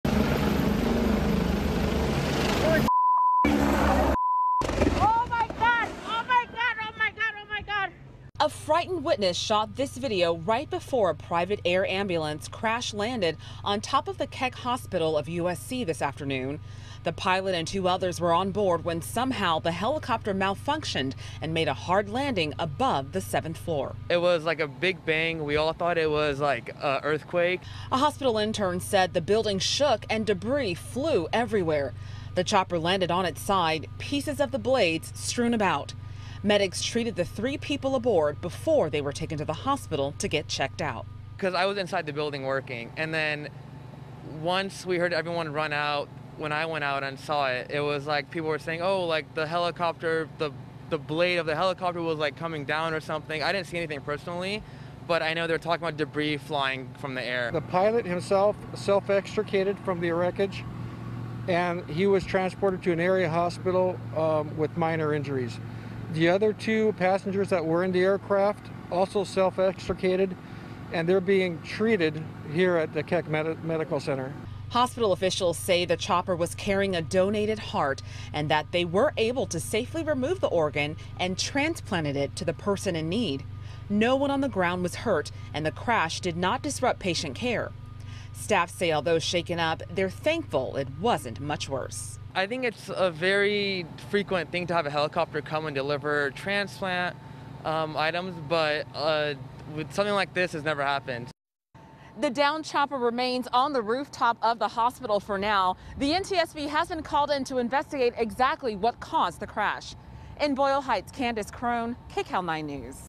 Witness Captures Video Moments Before Helicopter Crashes At Keck Hospital Of USC
Kandiss Crone reports.